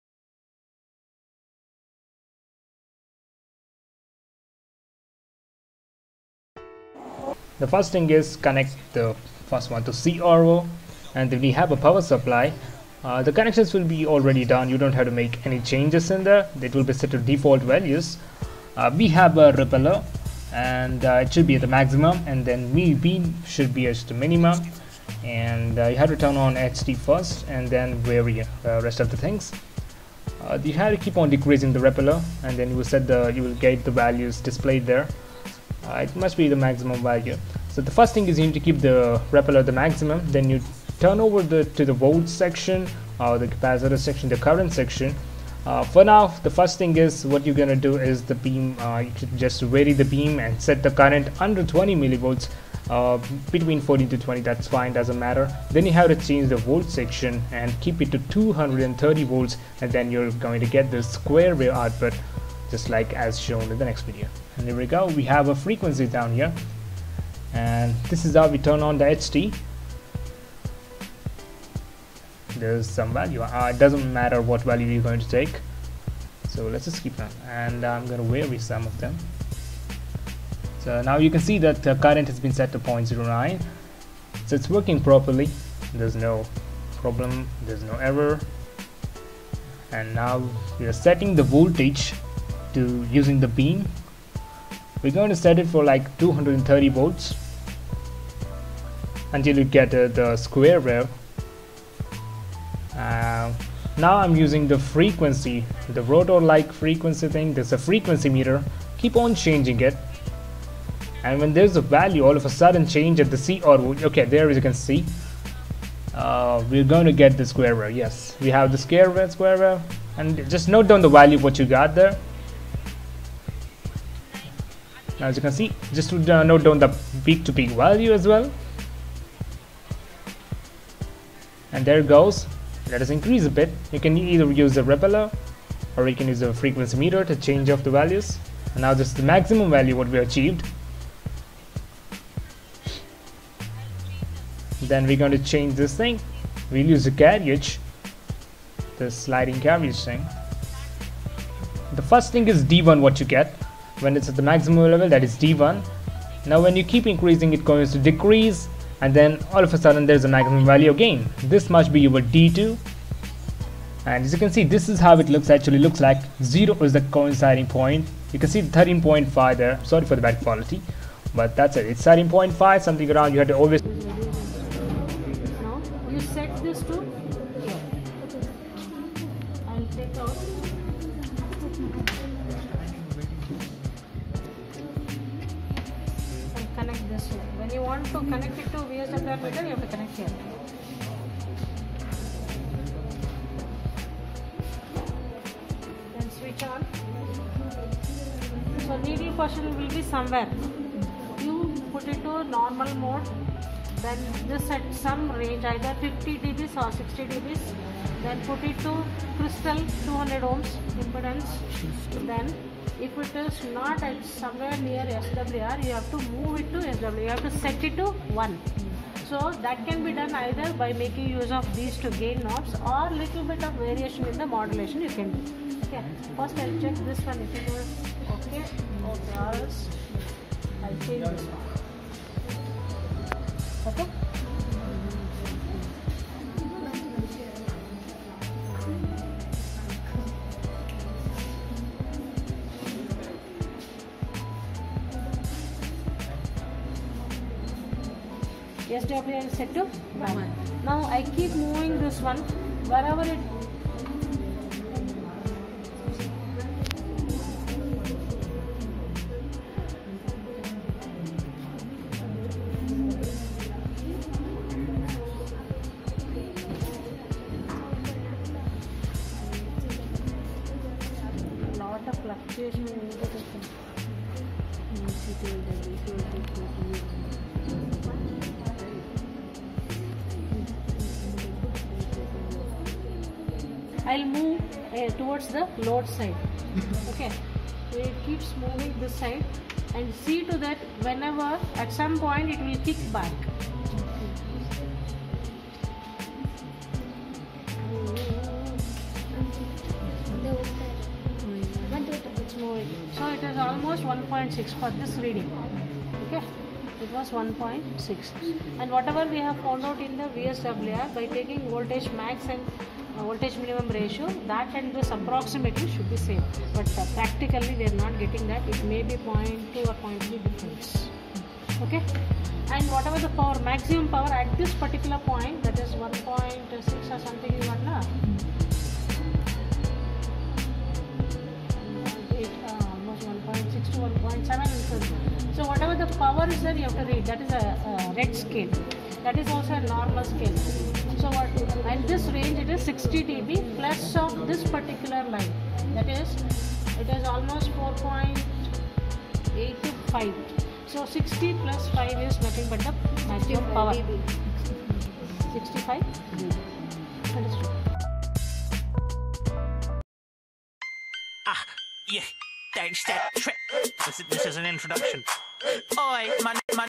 ADC Lab VTU - Measurement of Frequency, Wavelength, Power, Vswr, Microwave test bench(Klystron)
ADC Lab VTU Part A - Measurement of Frequency, Guide wavelength, Power, Vswr and Attenuation using Microwave test bench(Klystron)

All vtu lab experiments will be uploaded in this channel.
I expect nothing but Liking my videos and Subbing to my channel